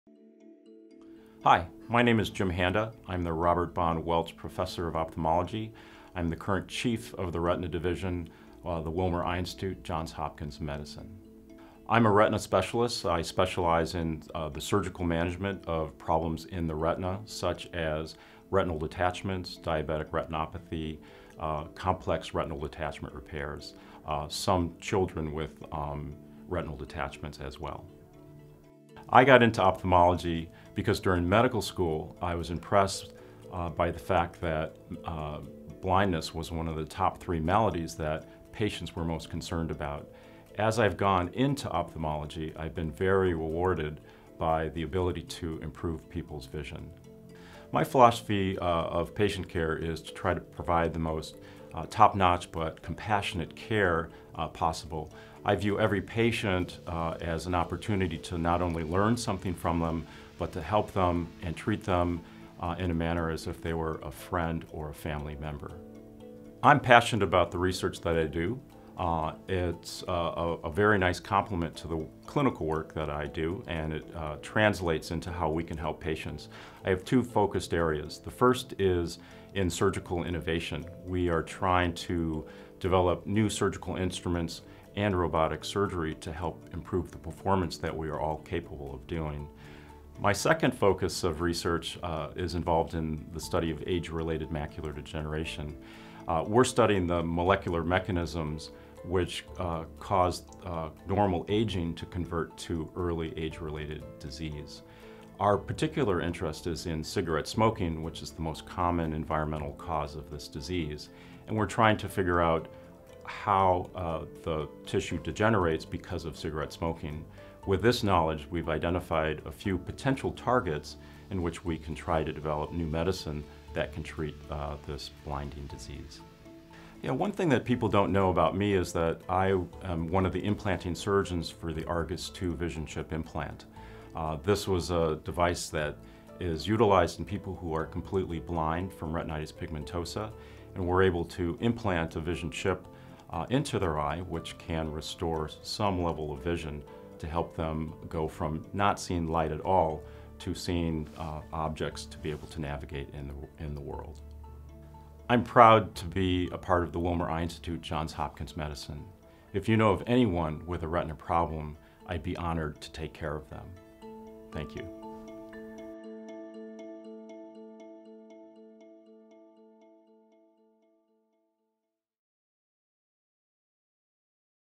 Dr. James Handa | Retina Ophthalmology
James T. Handa, M.D., is Chief of the Retina Division and the Robert Bond Welch, M.D., Professor of Ophthalmology at the Wilmer Eye Institute. He specializes in medical and surgical management of complex vitreoretinal diseases such as age-related macular degeneration, diabetic retinopathy, retinal detachment, retinopathy of prematurity and other pediatric retinal diseases. He also has expertise in intraocular oncology and manages patients with choroidal melanomas and metastatic tumors of the eye.

Not only is he a highly skilled surgeon and clinician, but he also devotes significant effort to research related to the early causes of age-related macular degeneration using molecular pathological approaches to understand how the eye transforms from normal aging to early disease.

He has been funded by the National Eye Institute for the last 19 years, and he currently holds two R01 awards for his work in AMD. He has also been funded by the Thome Foundation, Research to Prevent Blindness, Fight for Sight and the American Health Assistance Foundation. His other research focuses on surgical innovation using an integrated robotic surgical system. Dr. Handa is currently the Wilmer Eye Institute’s implanting surgeon for the Argus II retinal chip implant, which was recently approved by the FDA after Dr. Handa participated in the clinical trial leading to its approval.